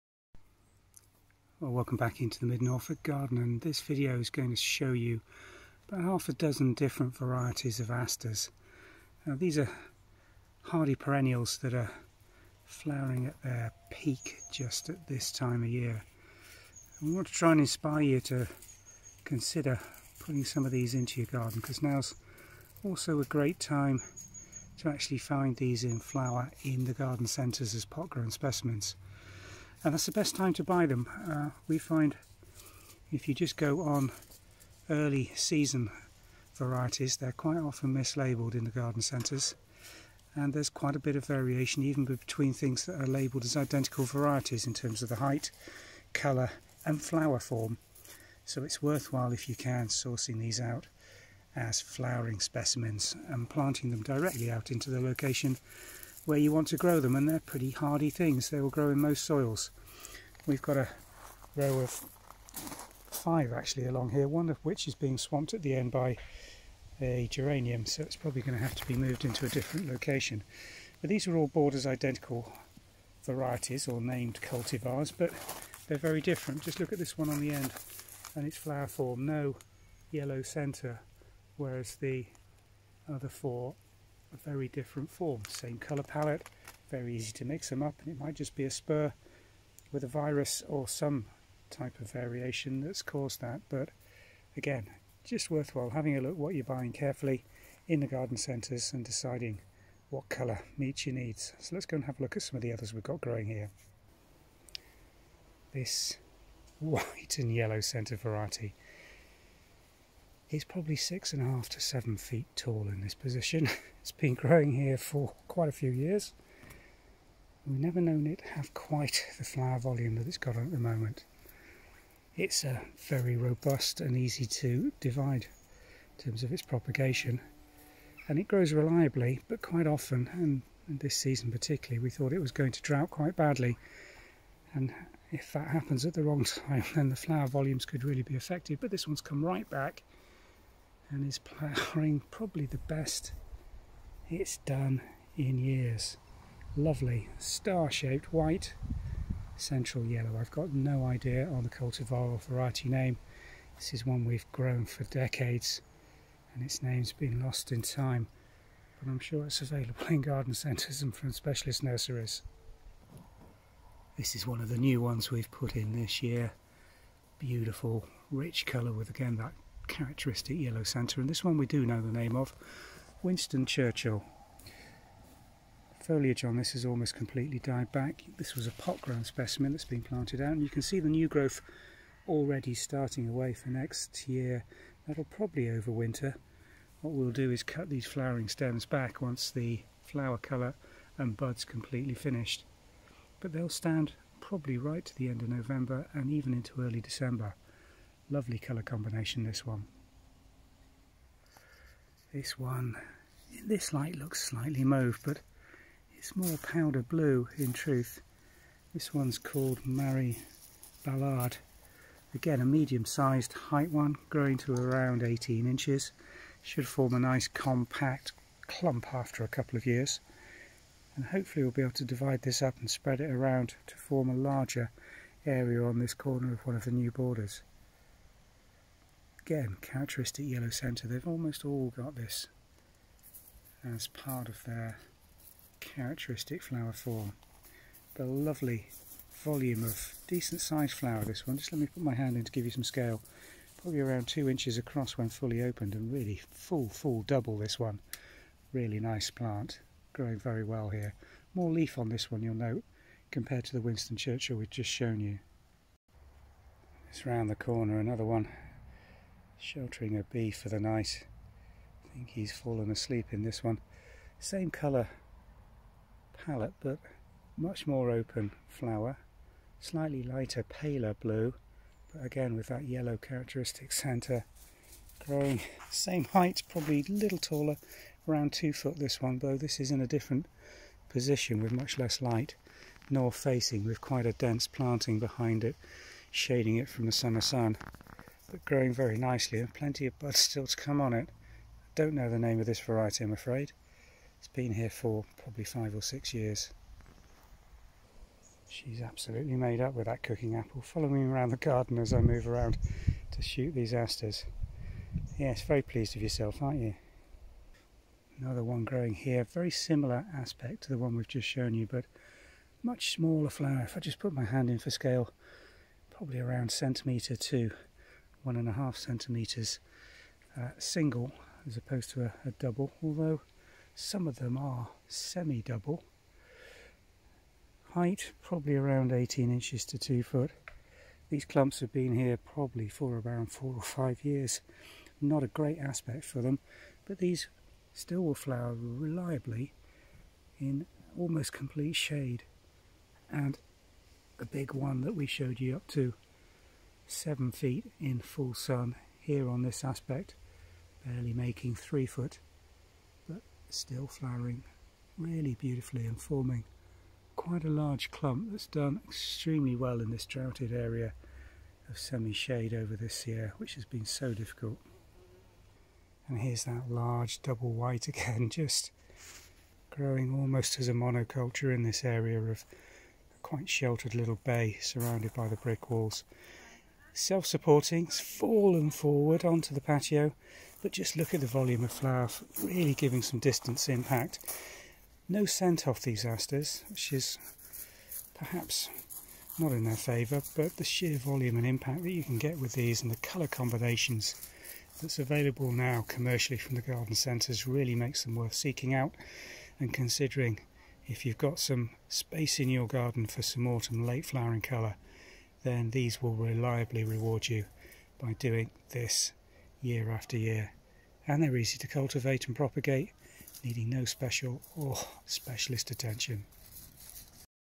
Grow Asters for late Garden Colour
Asters are really undervalued garden perennials for reliable late summer colour in the garden. In this video, we show a variety of Asters grown in our garden in mid-Norfolk. Most of the varieties will be available in garden centers and nurseries in flower now, and its a great time to consider buying and planting Asters in their final growing positions.